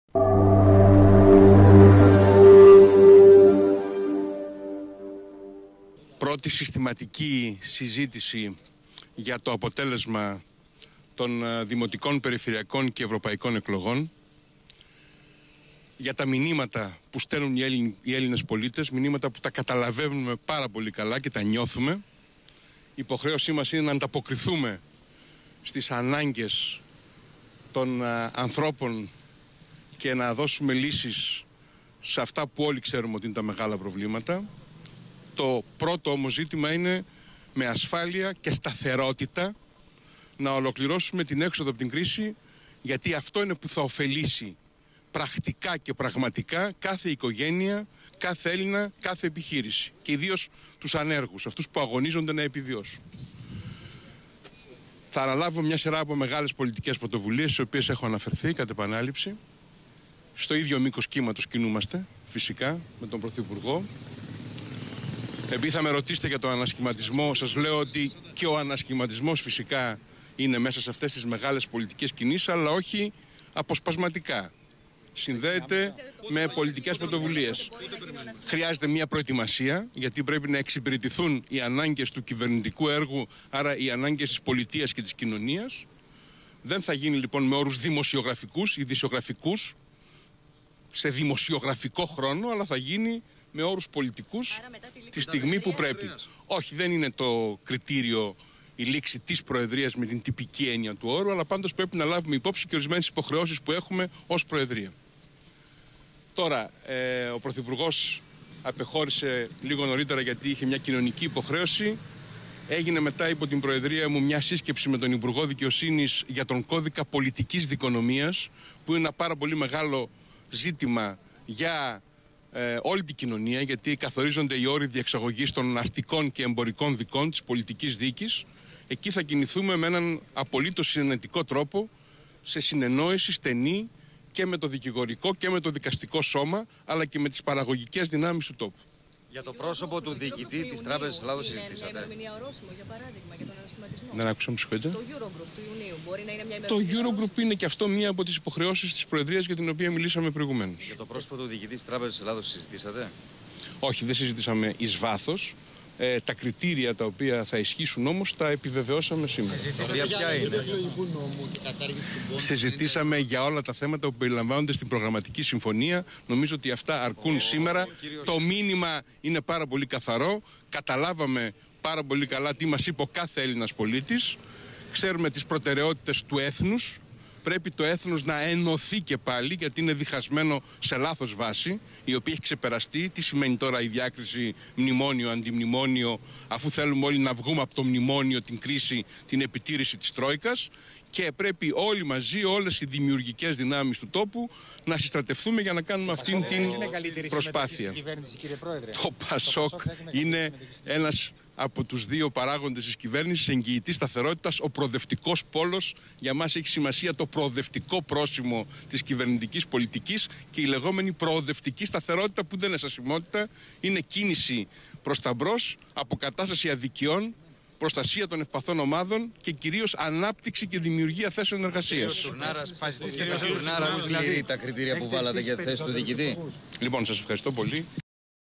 Ε.Βενιζέλος:Υποχρέωση μας να ανταποκριθούμε στα μηνύματα της κάλπης και να δώσουμε λύσεις
«Τα μηνύματα  των πολιτών  από τις κάλπες   τα καταλαβαίνουμε και τα νοιώθουμε Υποχρέωση μας να ανταποκριθούμε και να δώσουμε λύσεις.» Αυτό τόνισε ο Πρόεδρος του ΠΑΣΟΚ και αντιπρόεδρος της κυβέρνησης κ Ε. Βενιζέλος, μετά την συνάντηση του με τον πρωθυπουργό Α Σαμαρά, στο Μ Μαξίμου

Θα αναλάβω μεγάλες πολιτικές πρωτοβουλίες, είπε ο κ Βενιζέλος, ο οποίος για το θέμα του ανασχηματισμού τόνισε ότι δεν θα γίνει «σε δημοσιογραφικούς χρόνους» αλλά με πρόγραμμα και όχι αποσπασματικά.